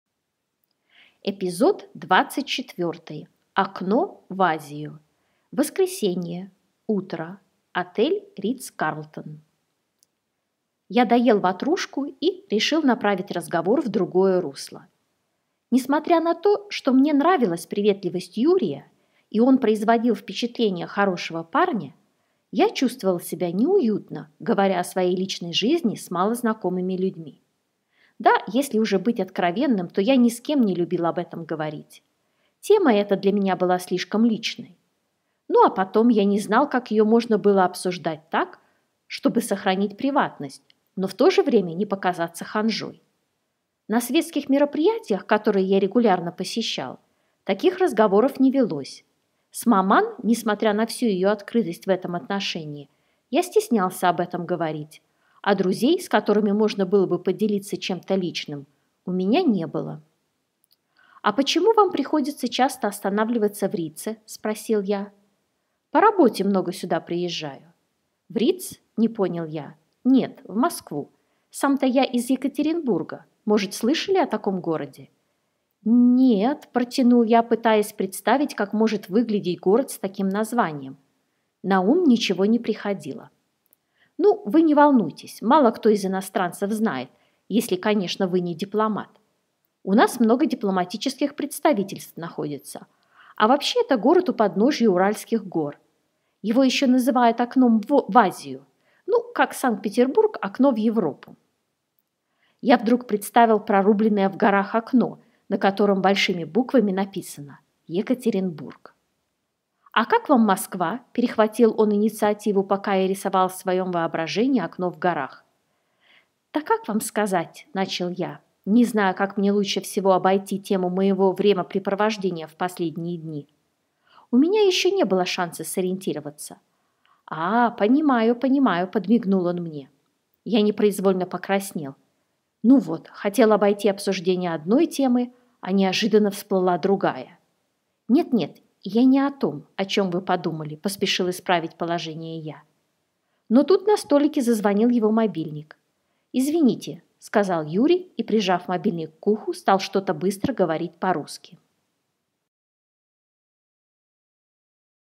Аудиокнига Задача с неизвестными - эпизод 24
Аудиокнига Задача с неизвестными
Эпизод 24 - «Окно» в Азию
Льюк продолжает разговор с Юрием и узнает о Екатеринбурге...

Цифровую и печатную версии книги Задача с неизвестными можно приобрести на следующих сайтах: